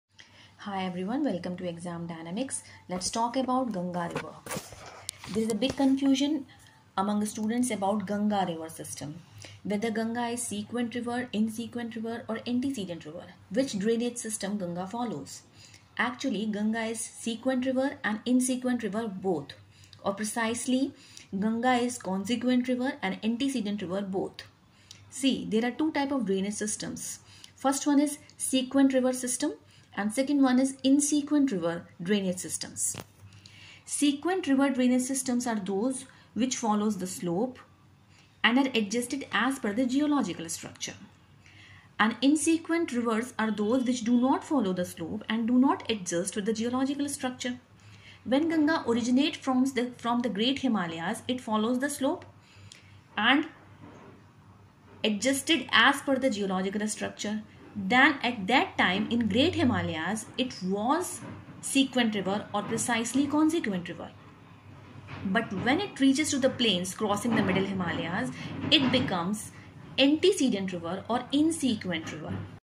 Ganga Drainage system: Sequent/ Insequent???
Plateau series in India Map
Allah Bund : Dam of God

Tropic of Cancer & Africa continent

Vanilla Islands in Indian Ocean